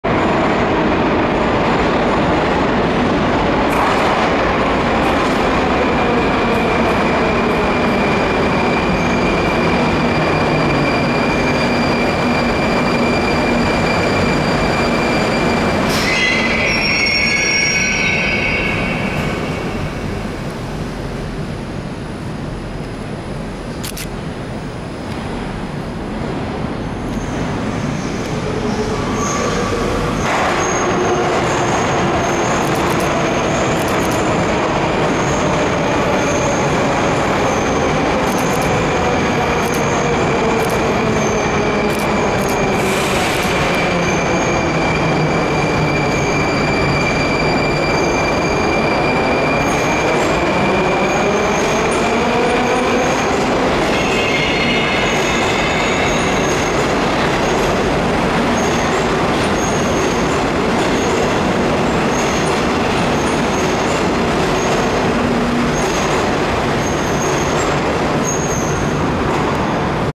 Тематический Поезд «Кино Победы» на станции Партизанская
Московский Метрополитен 81-740/741 серии 0097 и 0096 на станции Партизанская